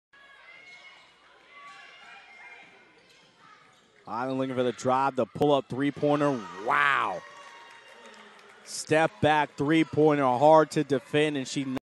Lake Highland Drains a 3 to End 1st Half
The highlanders nail a pull-up jumper from beyond the arc to bring their lead to 28-20 at the half.